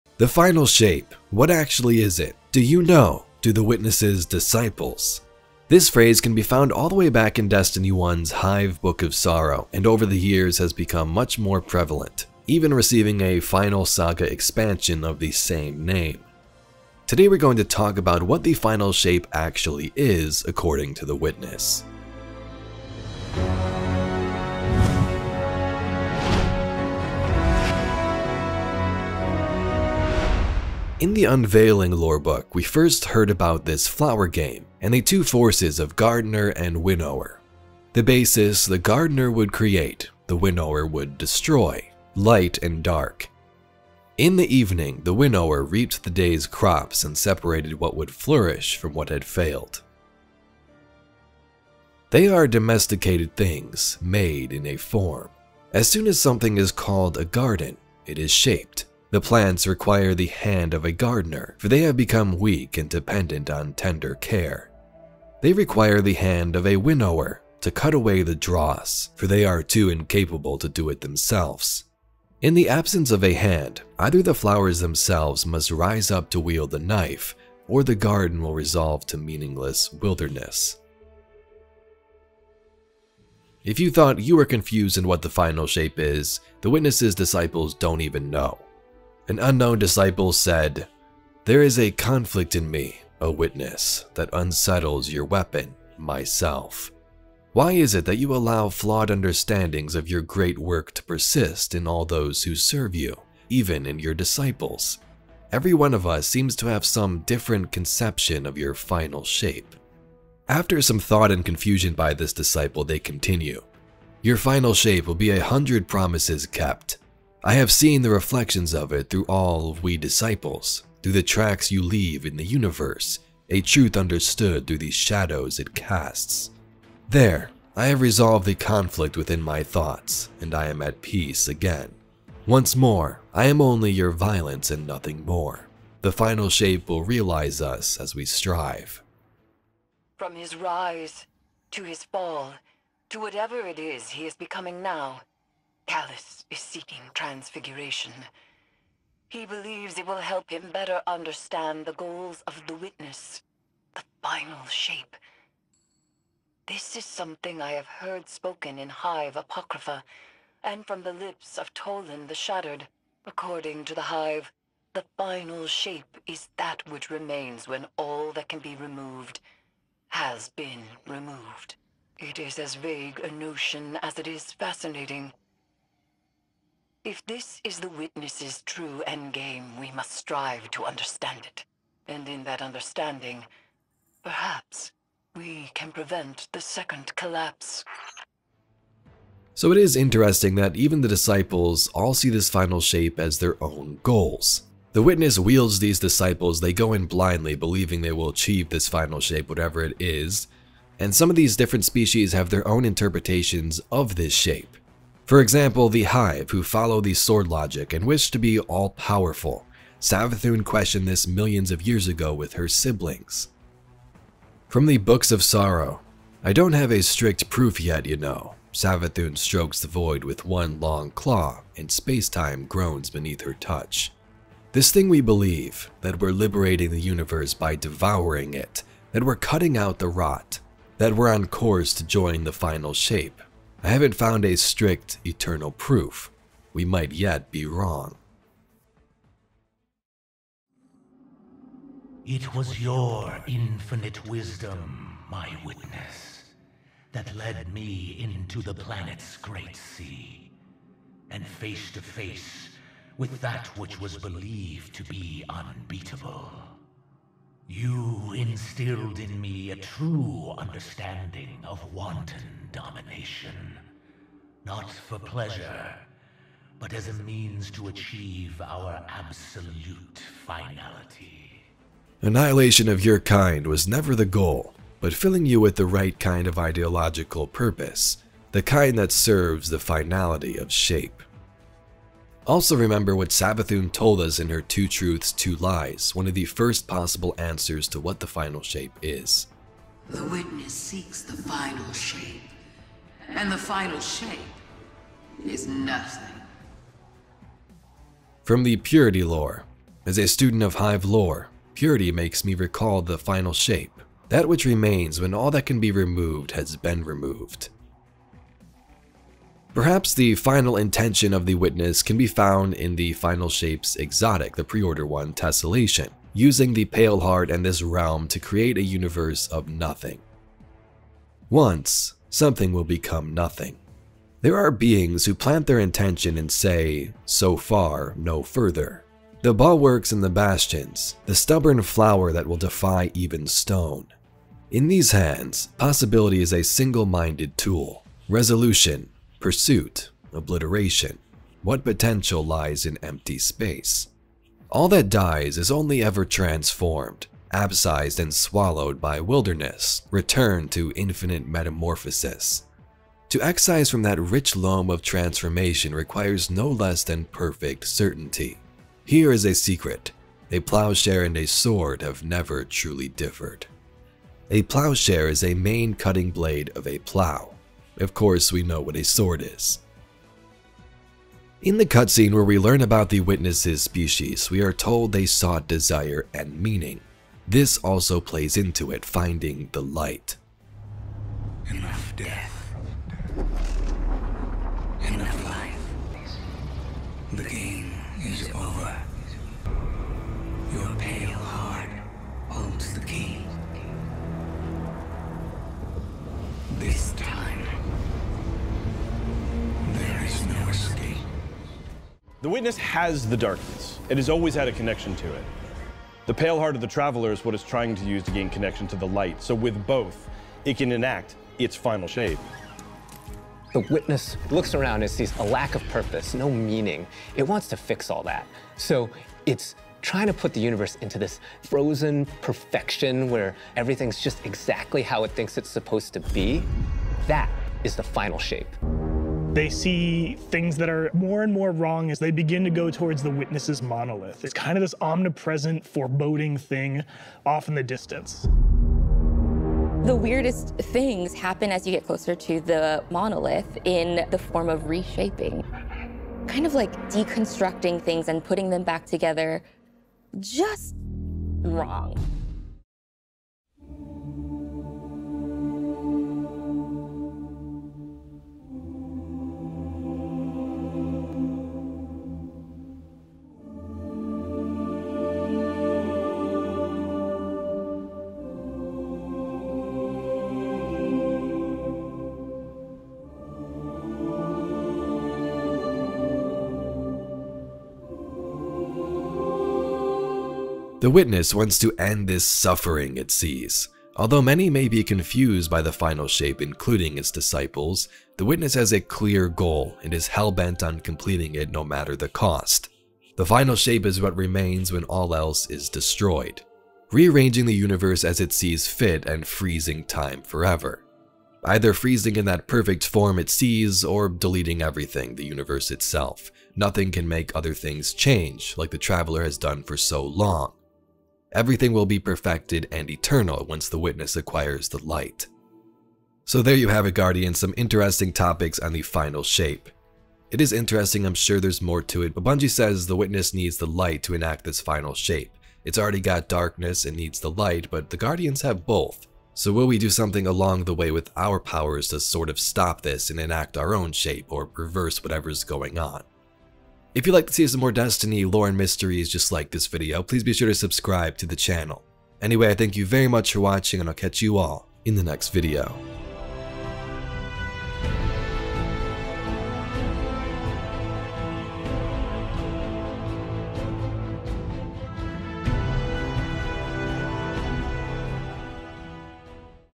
Destiny 2 - WHAT THE FINAL SHAPE ACTUALLY IS!
The Final Shape. What actually is it? Do you know? Do the Witness’ Disciples? This phrase can be found all the way back in Destiny 1’s Hive Book Of Sorrow and over the years has become more prevalent, even receiving a final saga expansion of the same name. Today we are going to talk about what the Final Shape actually is according to the Witness.

“Destiny 2 The Final Shape”
“Destiny 2 What Is The Final Shape”
“Destiny 2 Lore and Story”